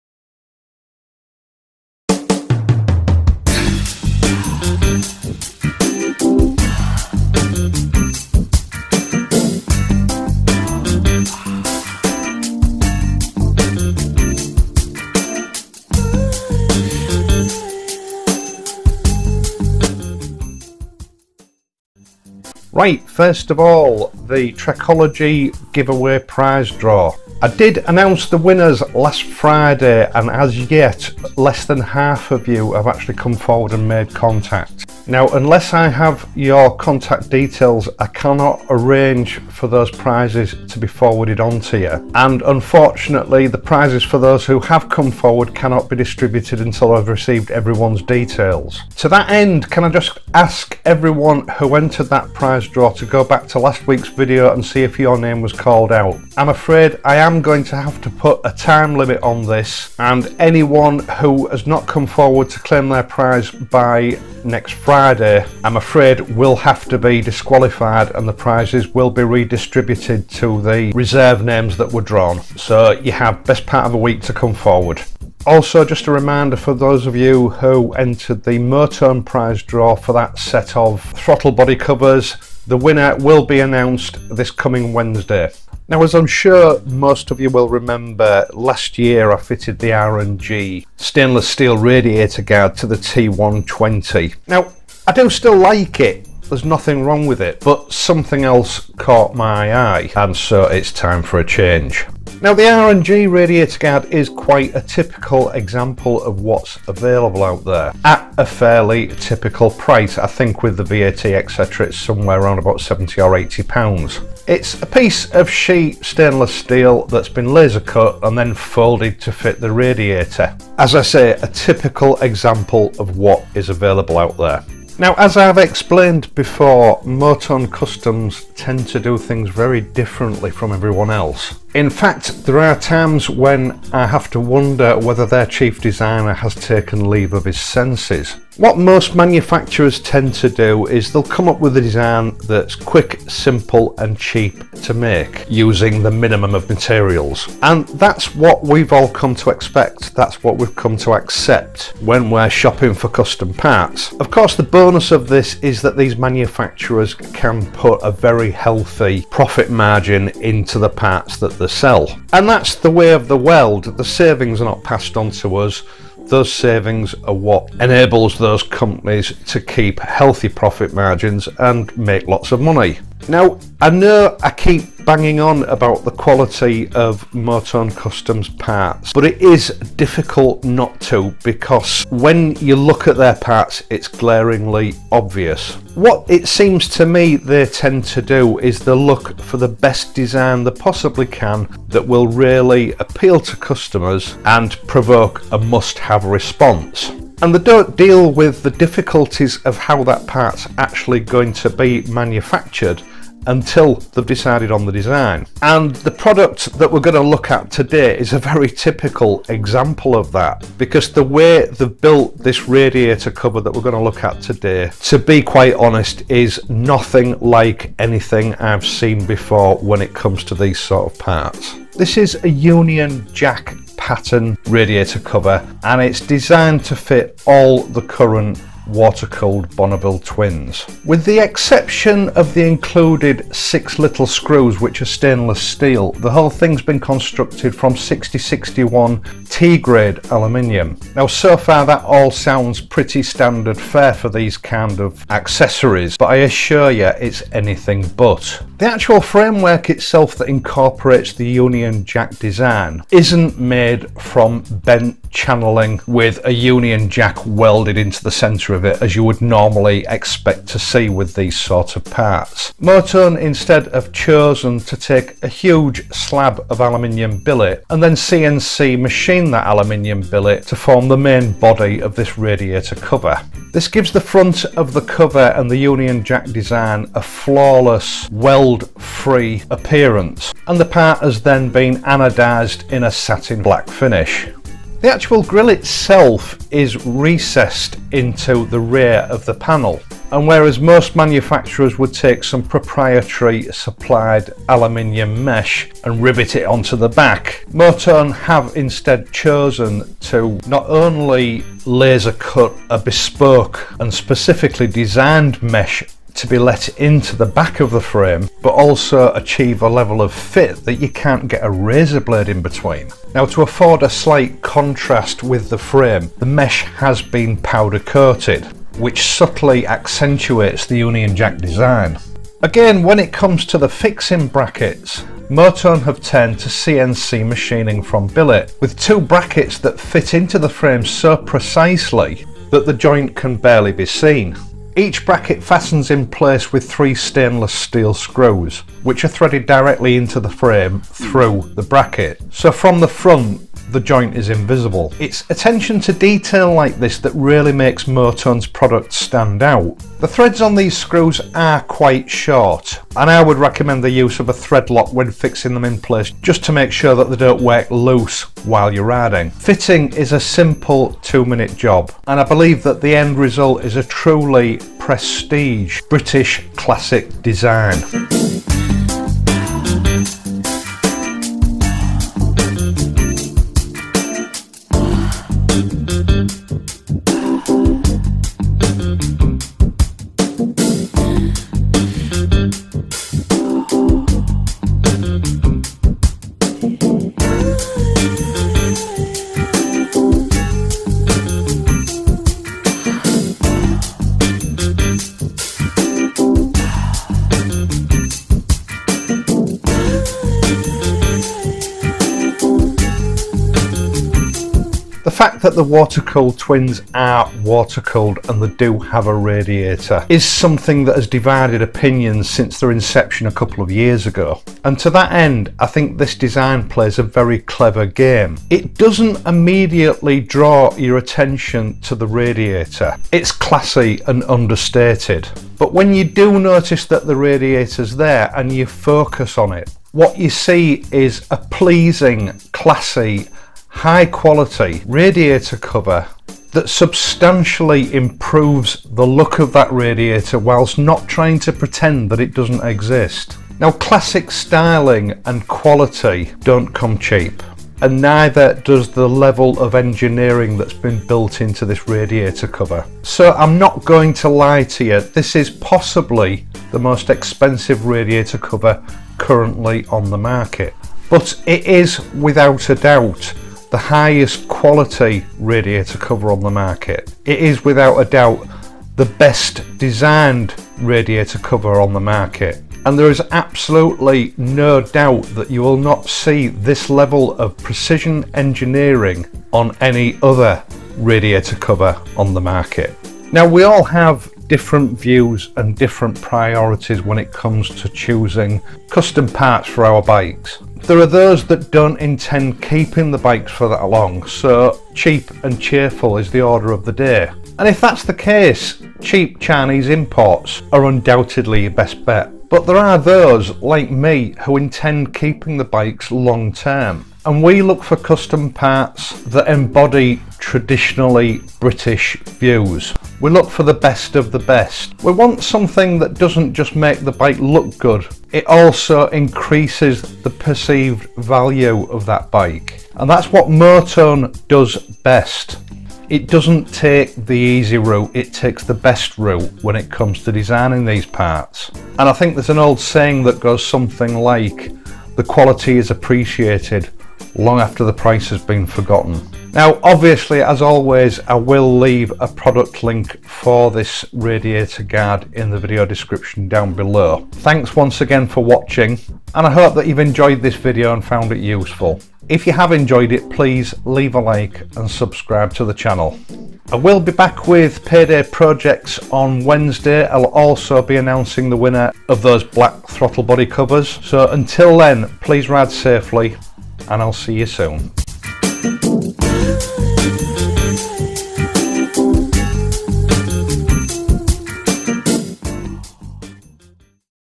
Triumph Bonneville T120/RULE BRITANNIA! ultimate classic Union Jack Radiator guard/cover
A look at Motones new ultra high quality Radiator guard for the Triumph T100, Street Twin, Street cup, street Scrambler, Thruxton, etc!